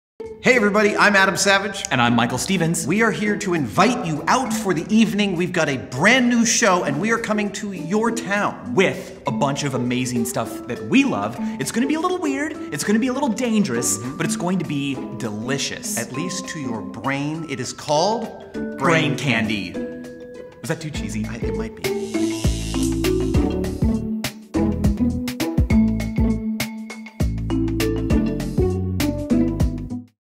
Brain Candy YouTube 30
Brain Candy, with Adam Savage and Michael Stevens, comes to Popejoy Hall March 31. They're bringing a truckload of toys to answer some very interesting questions.